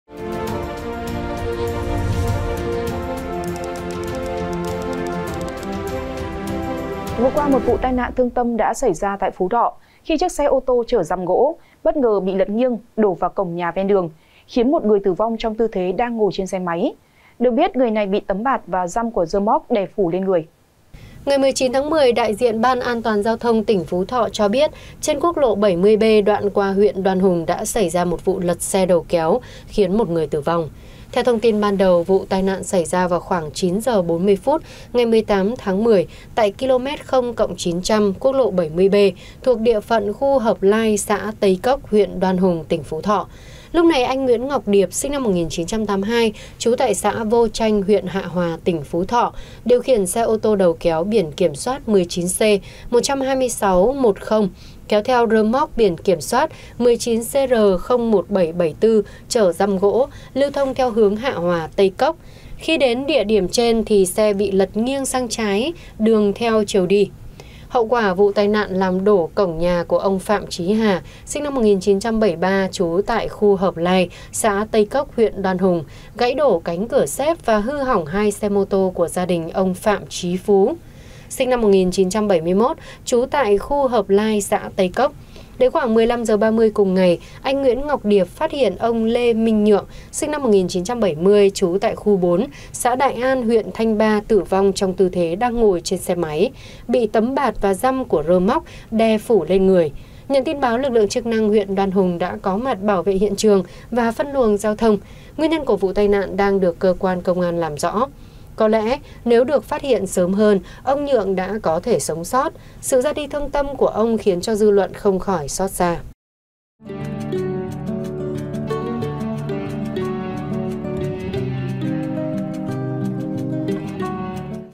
Tá Hỏa Phát Hiện Người Đàn Ông Tử Vong Trong Tư Thế Lạ Dưới Đống Dăm Gỗ Ở Phú Thọ | SKĐS
SKĐS | Vừa qua, một vụ tai nạn thương tâm đã xảy ra tại Phú Thọ khi chiếc xe ô tô chở dăm gỗ bất ngờ bị lật nghiêng đổ vào cổng nhà ven đường, khiến một người tử vong trong tư thế đang ngồi trên xe máy. Được biết, người này bị tấm bạt và dăm của rơ moóc đè phủ lên người.

Mời quý vị và các bạn xem thêm: Nữ Sinh Xưng “Mày - Tao” Với Thầy Giáo Bị Đình Chỉ Học 1 Tuần, Có Hoàn Cảnh Gia Đình Đặc Biệt